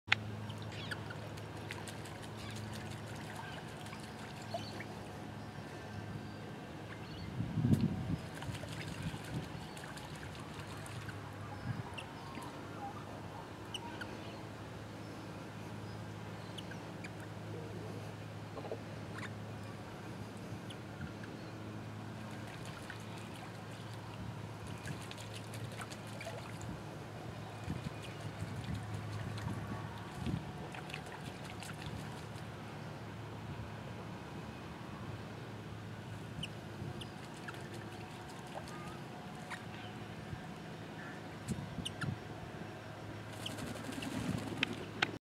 Twin Bathing
Crimson Rosellas enjoying a morning bath